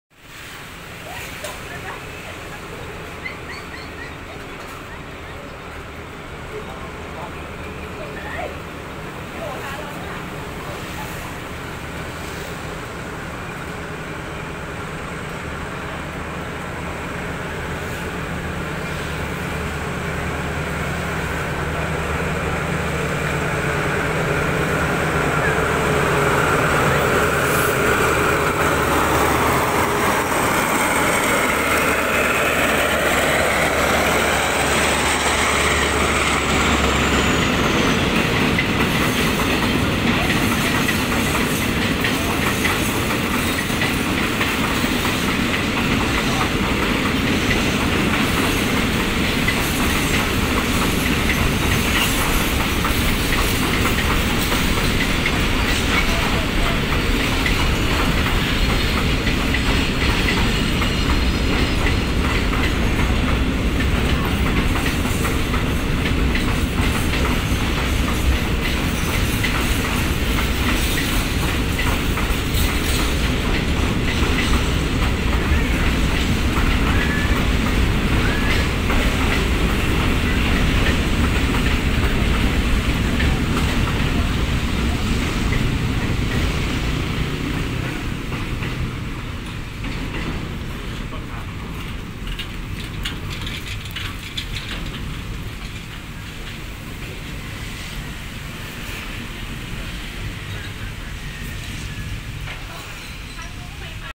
SRT. ขบวนรถสินค้าที่ 2126 แหลมฉบัง-ชุมทางบางซื่อ ผ่านสถานีชุมทางบางซื่อในชานชาลาที่ 3
Facebook : SRT Thai Railfanning - คนรักรถไฟ